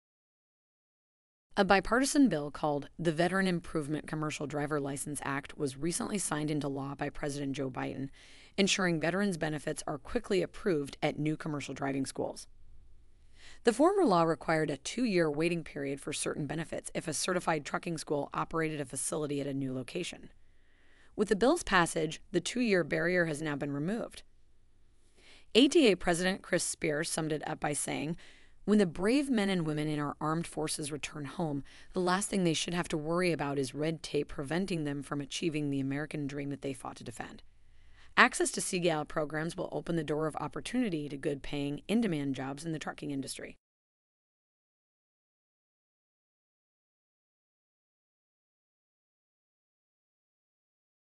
Biden: Law Removes Barriers for Veterans at New CDL School Branches
President Joe Biden on Oct. 1 signed into law a bill aimed at ensuring veterans’ benefits are approved without delay at new commercial driving schools.